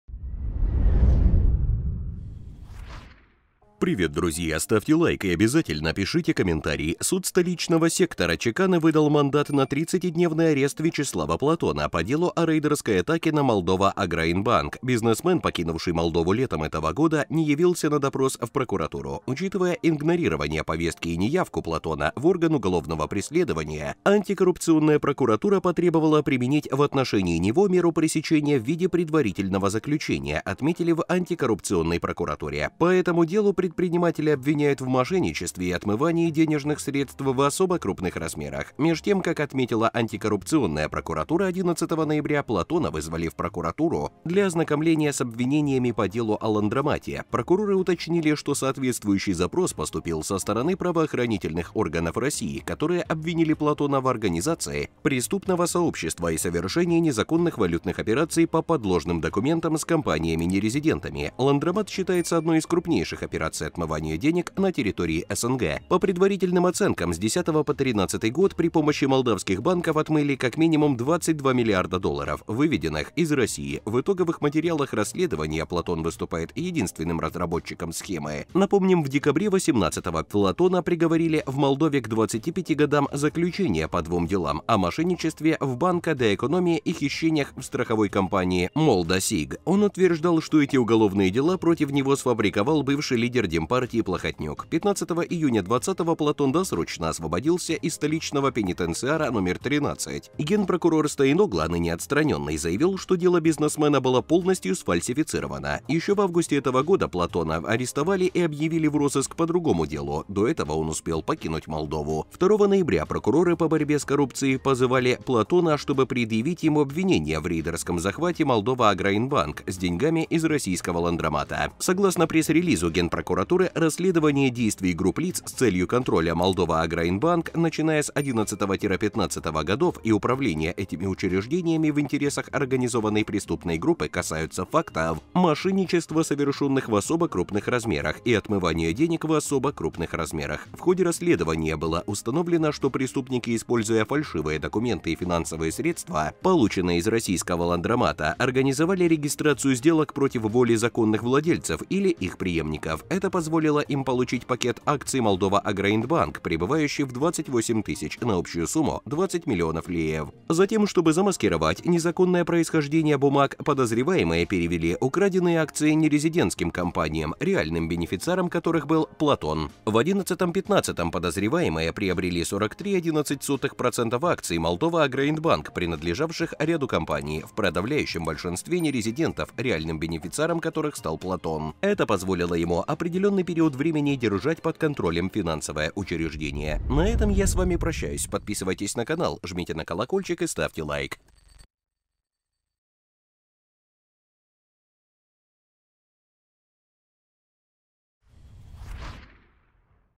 Срочно! Арест – Платон в истерике: посадят, рейдерская атака! Он не отмажется – украли миллионы
Вот о чём идёт речь:

Суд столичного сектора Чеканы выдал мандат на 30-дневный арест Вячеслава Платона по делу о рейдерской атаке на Moldova-Agroindbank. Бизнесмен, покинувший Молдову летом этого года, не явился на допрос в прокуратуру.